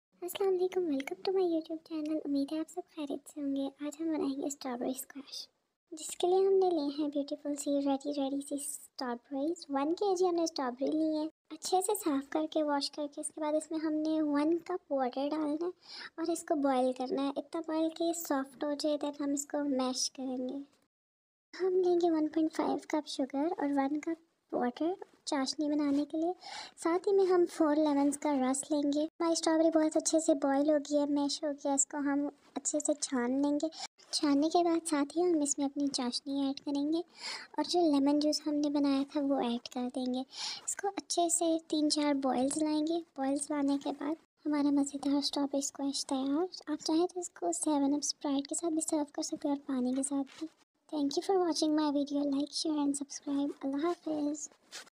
STRAWBERRY SQUASH BANAI AB 15 MINUTES MA 😋
yummy strawberry squash
 recipe
strawberry= 1 kg
water=1cup
sugar= 1.5 cup
lemon= 4